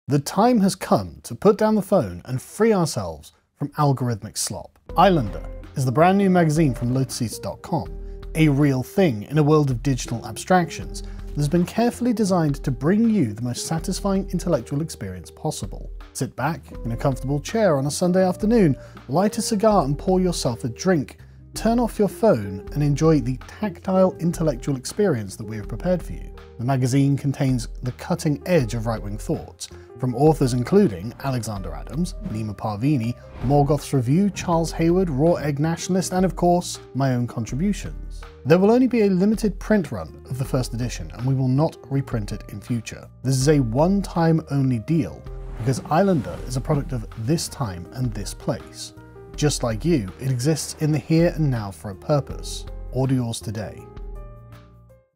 🏝️ Islander Magazine - Big Reveal 🏝️
PREORDER ISLANDER MAGAZINE NOW - FOR A LIMITED TIME ONLY
Dive into the vanguard of political commentary, cultural critique, and aesthetics with Islander, the premier print publication from lotuseaters.com. This inaugural issue features incisive articles from renowned voices such as Carl Benjamin, Dr Neema Parvini (the Academic Agent), Raw Egg Nationalist, Charles Haywood, Alexander Adams, and Morgoth's Review.